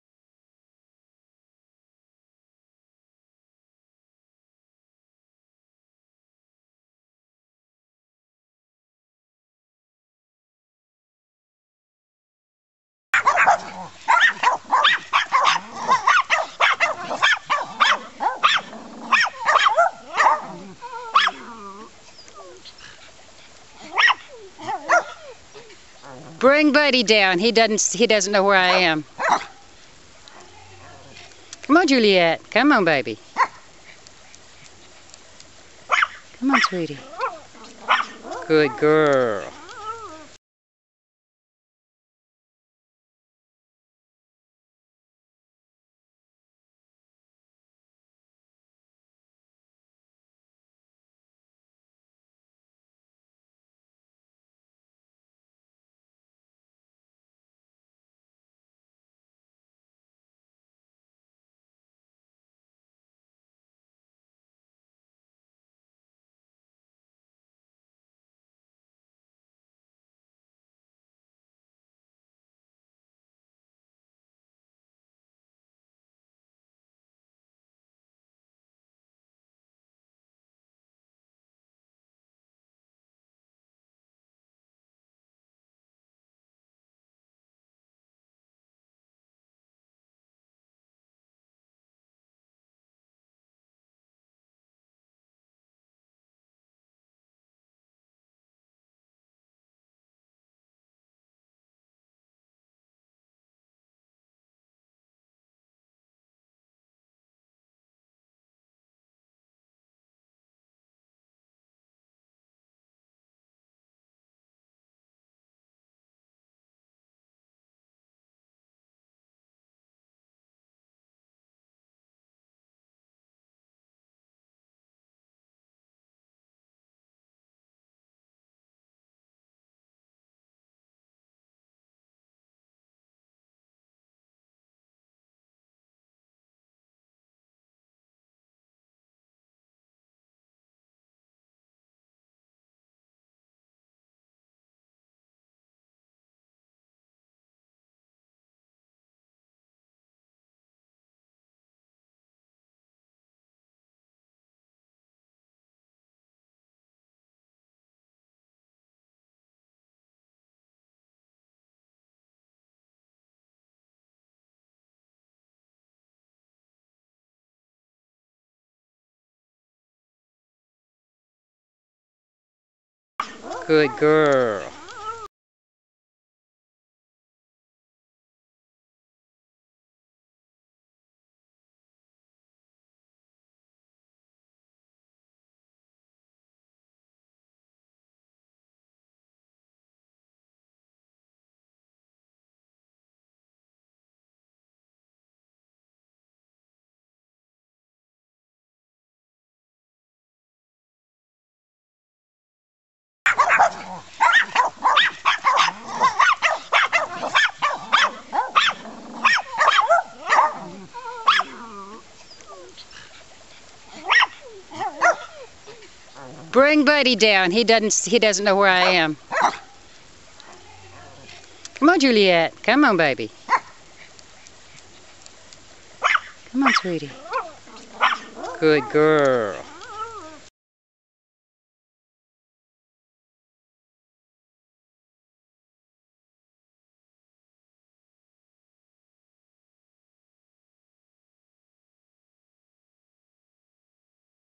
Juliette on the stairs 8 14 12
Juliette is almost 15 years old and is losing her sight. I had not noticed until today how bad it actually is because she, unlike humans, doesn't whine about it or let it stop her. She's such a brave little furkid.